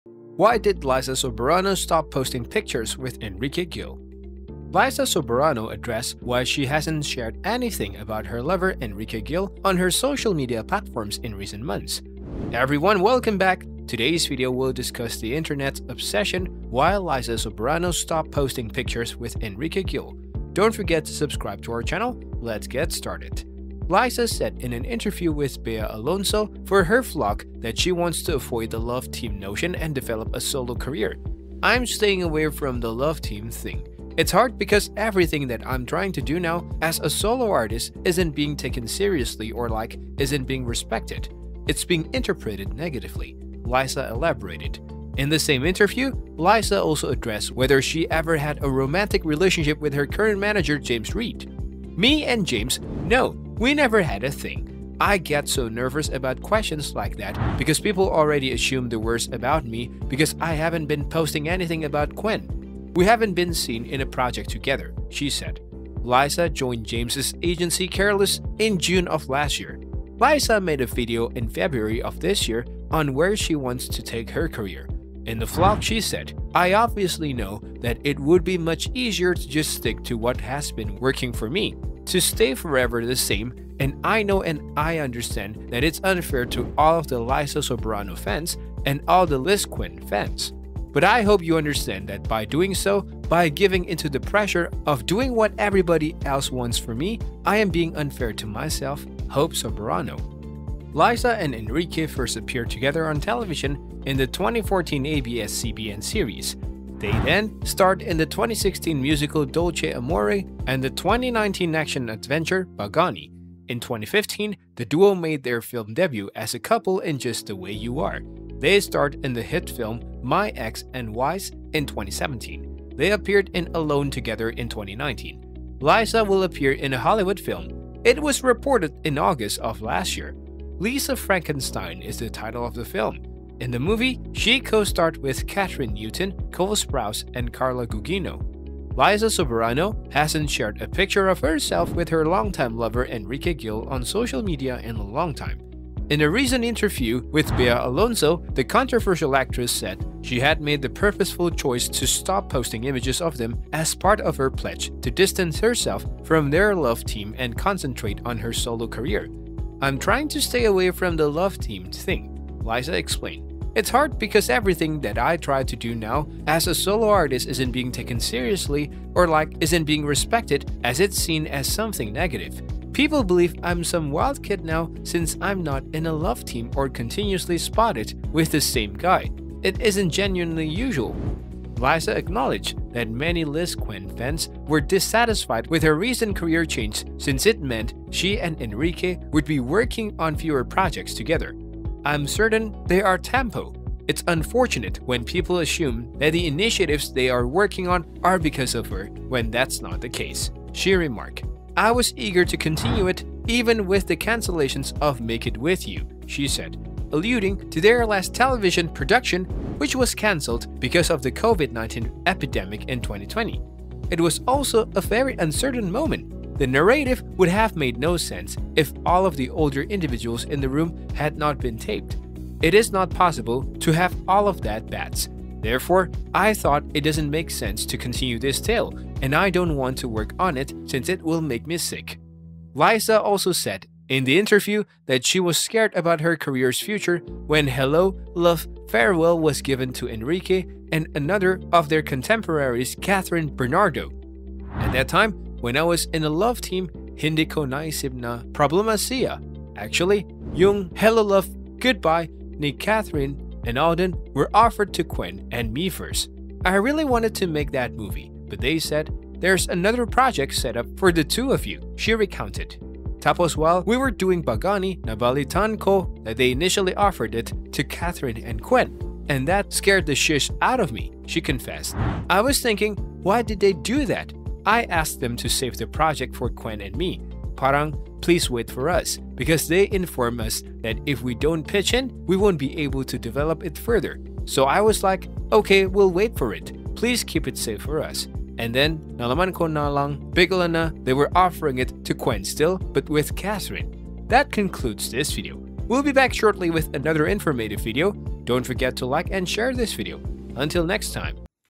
Why Liza Soberano STOPPED Posting Pictures With Enrique Gil!
Hello there, and welcome to CelebFest! Filipina-American actress, Liza Soberano, recently made the conscious decision of not posting pictures with her boyfriend and "love team" partner, Enrique Gil. This has stirred a great amount of controversy in the Philippine media as well as among fans. So in today's video, we will find out the real reason behind this.